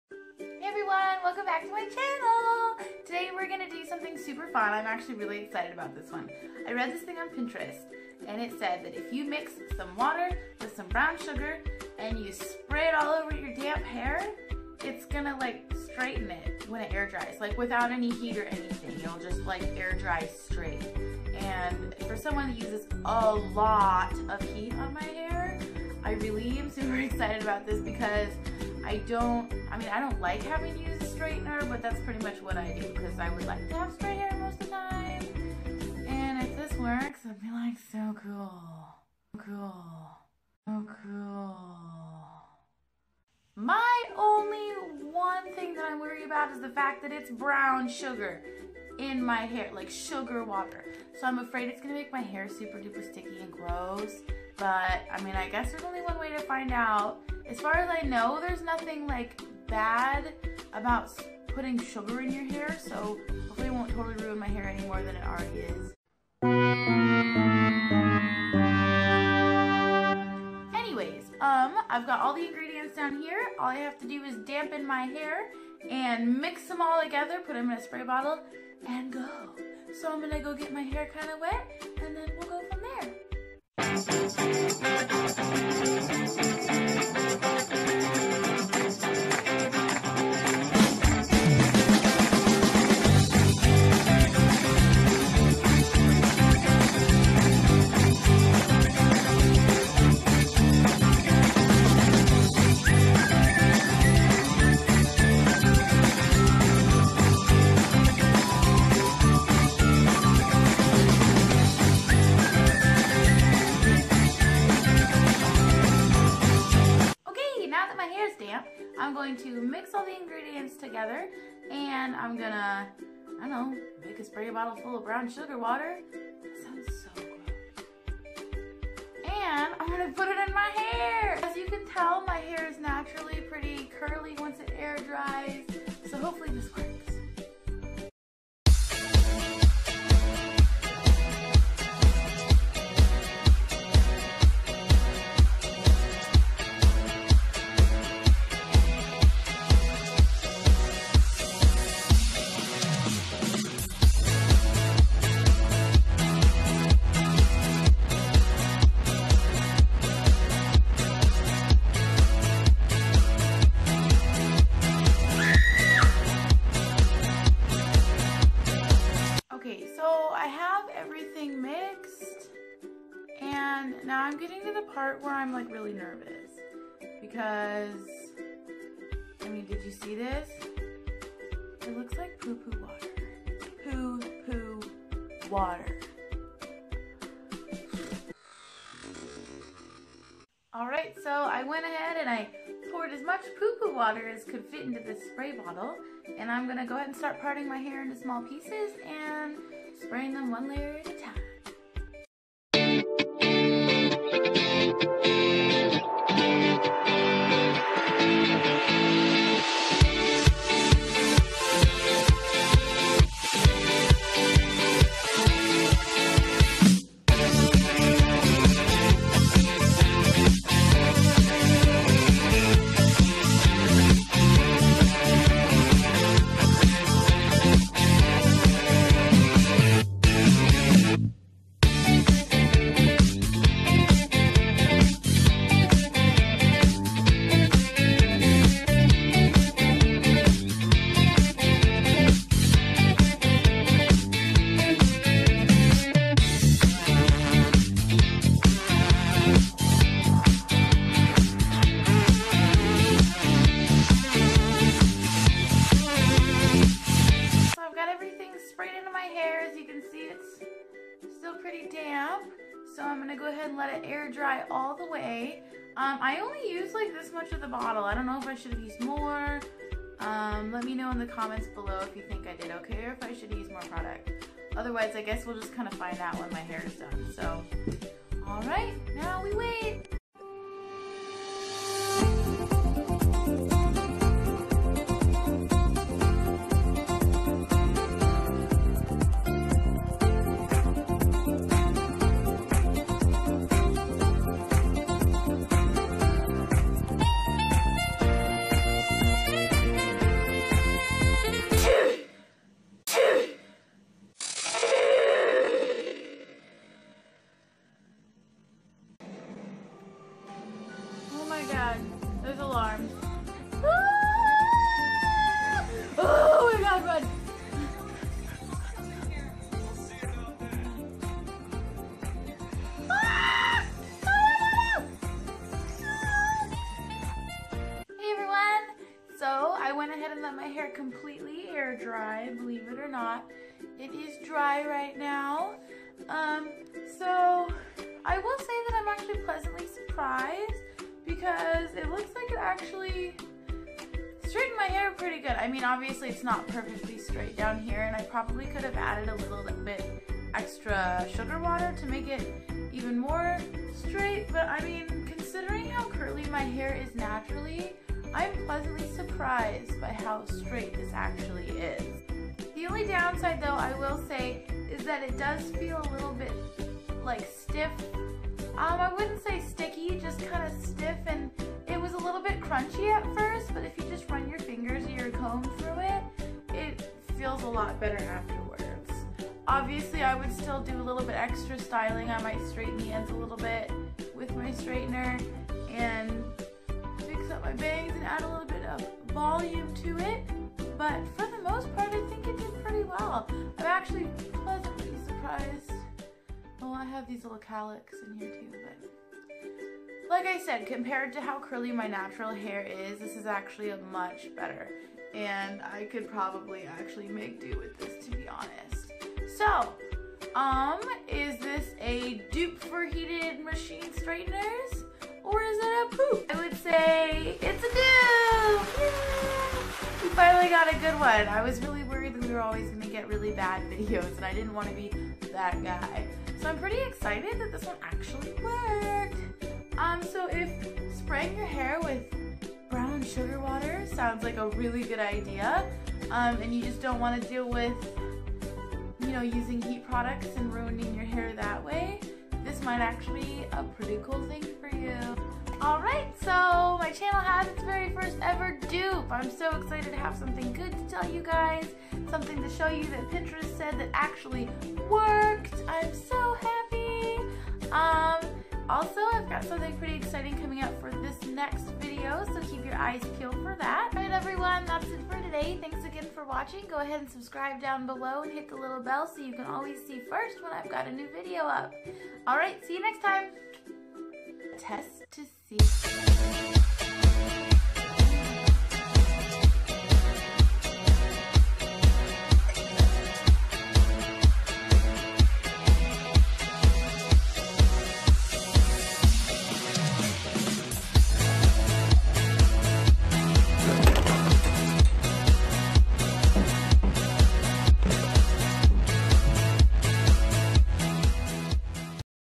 Does Sugar Water Make Hair Air Dry Straight?! DUPE or POOP?!
Thanks to a Pinterest post I found along with a few other beauty hacks on the same website, I've decided to try mixing brown sugar and water and spray it all over my damp hair! Apparently it's supposed to make your hair dry straight?

Only one way to find out!

Thanks lovelies!